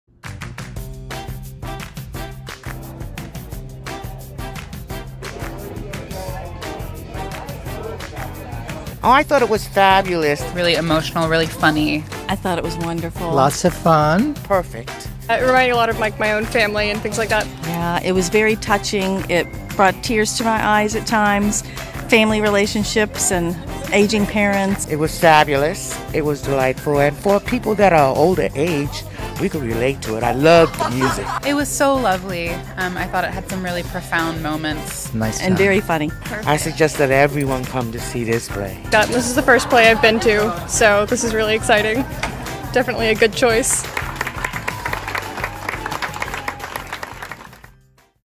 "Bonnie & Claire" Opening Night!
Audiences enjoyed Bo Wilson’s new funny and heartwarming story about two sisters who reluctantly reunite after a forty-year separation. Don't miss your chance to catch this world premiere! Visit virginiarep.org for tickets today.